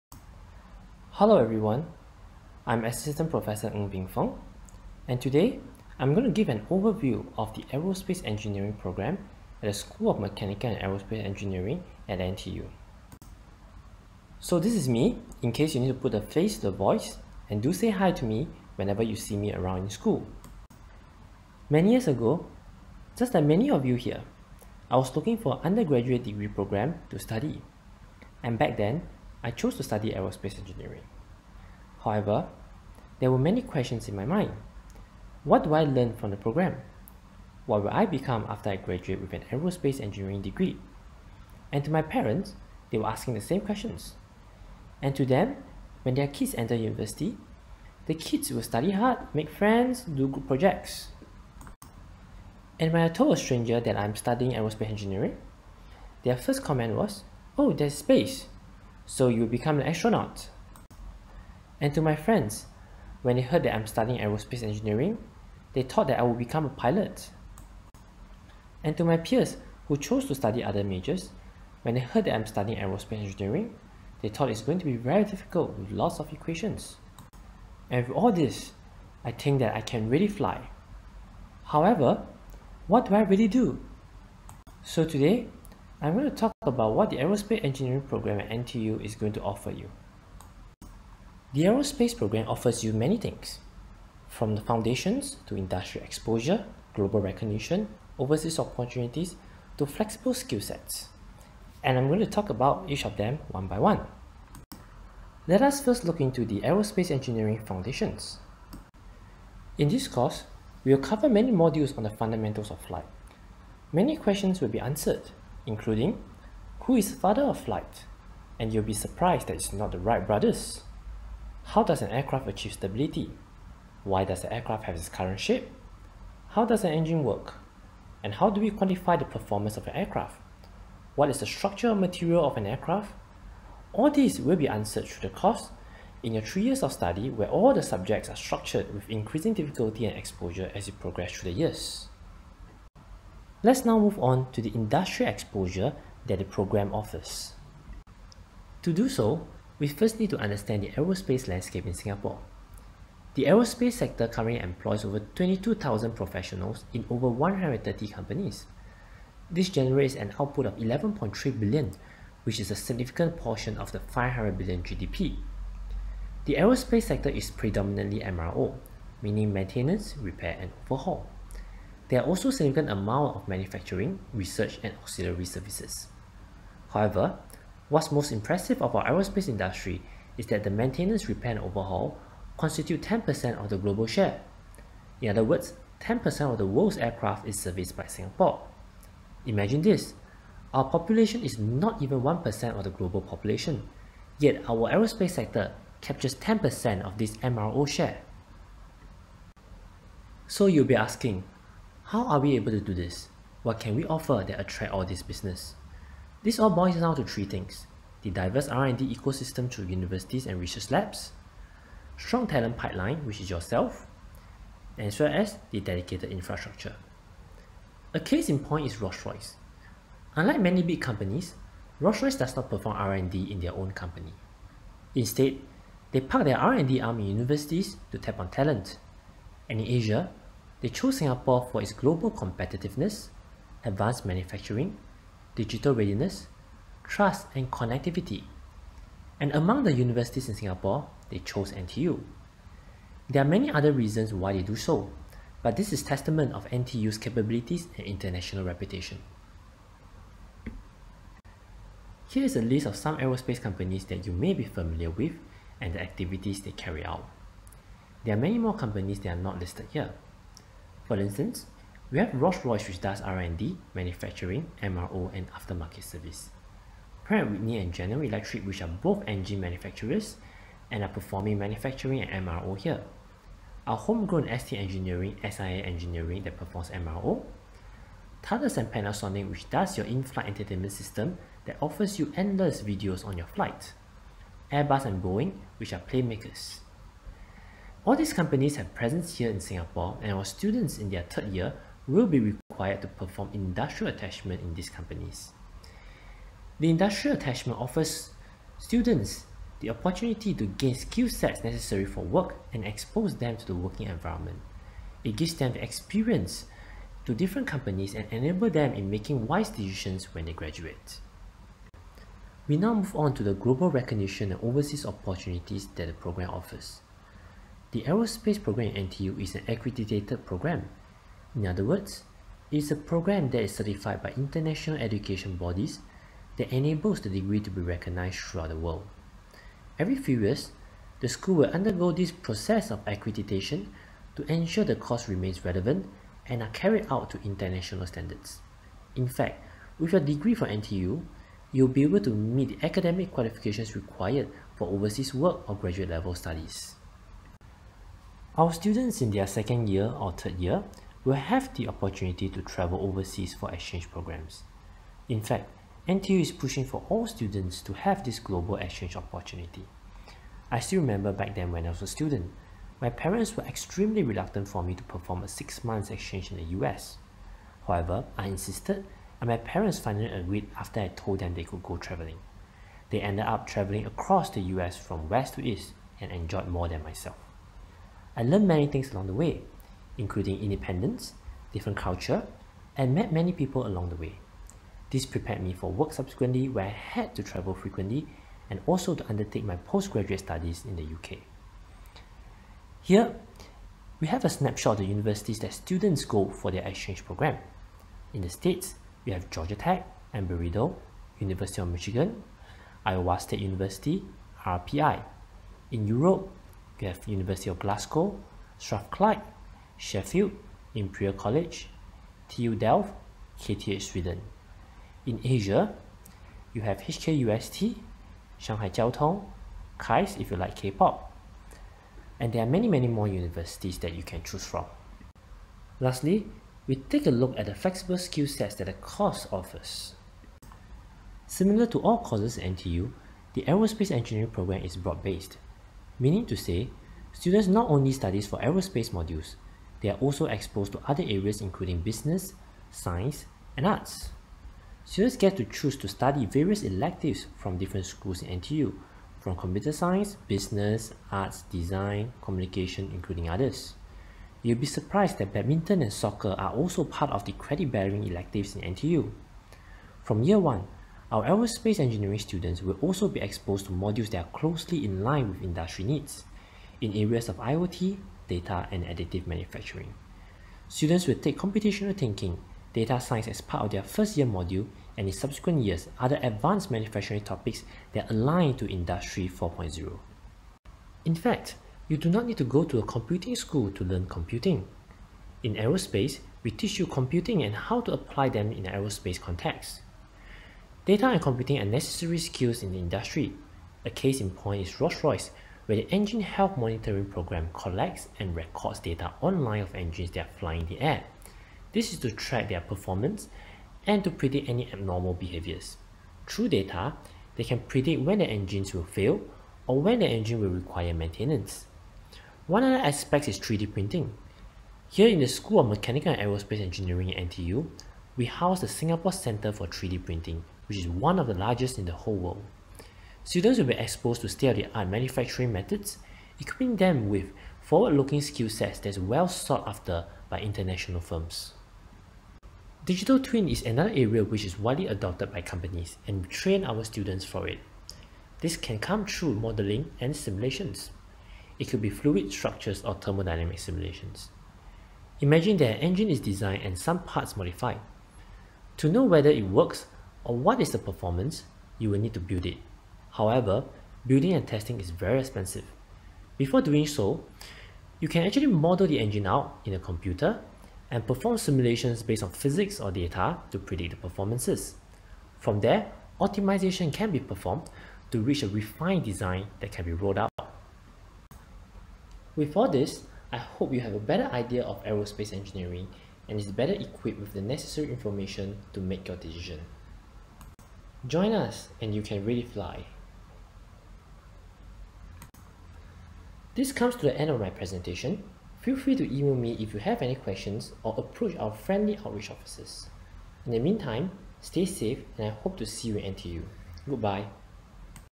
NTU MAE Aerospace Engineering
Why NTU MAE? Find out more about our Aerospace Engineering degree from Prof Ng Bing Feng!